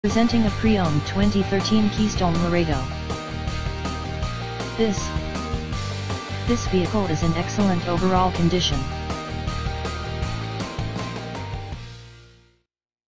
2013 Keystone LAREDO for sale in FORT MYERS, FL
JUST IN
SIMPLY GORGEOUS WITH BEAUTIFUL CABINETRY
1 AWNING
SLEEPS 4
ALUMINUM FRAME
REAR LADDER
REAR MOUNTED TIRE
BASE WEIGHT: 8396 LBS
HITCH WEIGHT:  1900 LBS
FRESH WATER:  55 GAL
DINETTE
OVERHEAD STORAGE
BUILT IN ENTERTAINMENT CENTER
FLAT SCREEN TV
SWIVEL CHAIRS (2)
2 A/C'S
SOFA
KITCHEN:
2 DOOR FRIG
STOVE
OVEN
MICROWAVE
CORNER DOUBLE SINKS
GOOSE NECK FAUCET
FOLD UP EXTENSION FOR COUNTERTOP
BATHROOM:
SHOWER
DRESSER
MEDICINE CABINET
BELOW SINK STORAGE
BEDROOM:
QUEEN BED
MIRRORED SLIDING DOOR
WARDROBE CLOSET
TRADE INS WELCOME AND FINANCING AVAILABLE
WE HAVE BEEN IN BUSINESS OVER 28 YEARS
RIGHT HERE IN FORT MYERS
CALL TOM FOR LOCATION AND APPOINTMENT